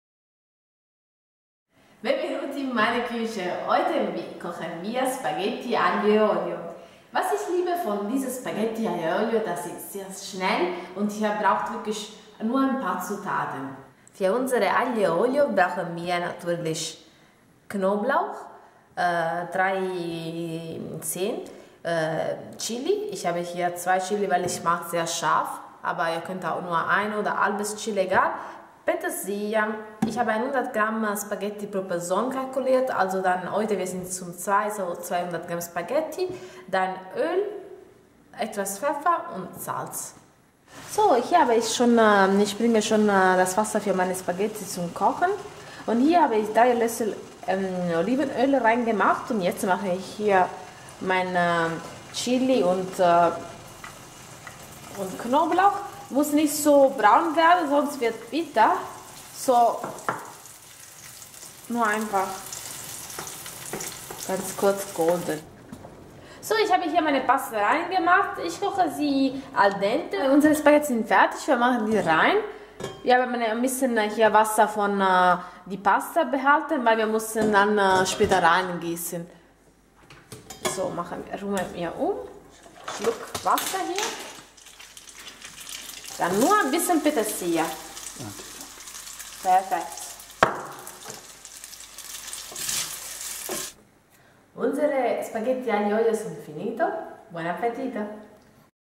Rezept für typische Spagehtti Aglio e Olio aus bella Italia
typisches italienisches Rezept für Spaghetti Agil e Olio. Ganz schnellen der Zubereitung und sehr lecker...Buon Appetito